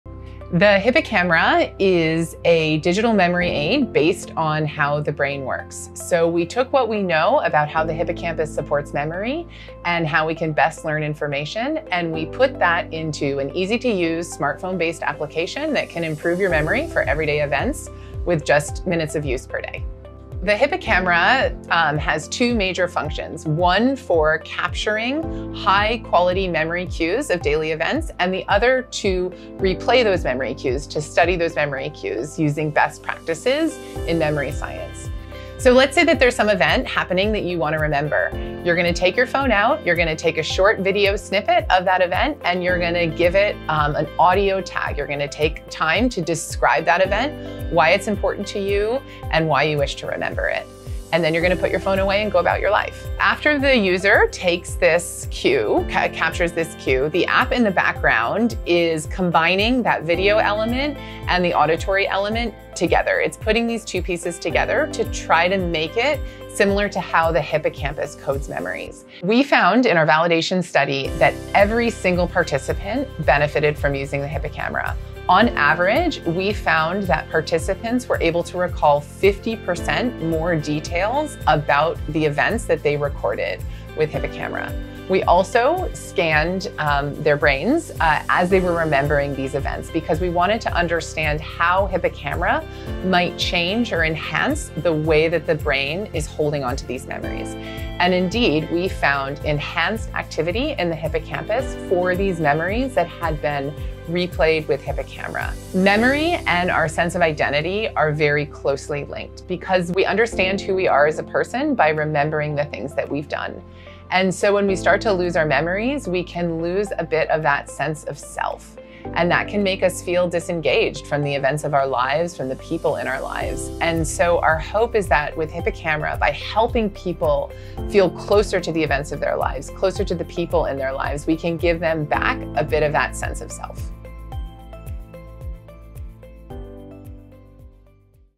Improving memory recall with HippoCamera app
University of Toronto neuroscientist Morgan Barense describes how the smartphone app HippoCamera being developed at U of T helps improve recall of memories that are richer in detail and more meaningful. With an easy-to-use interface, HippoCamera is a personalized way to boost recall of daily experiences and enhance activity in the hippocampus, a part of the brain which plays a key role in memory.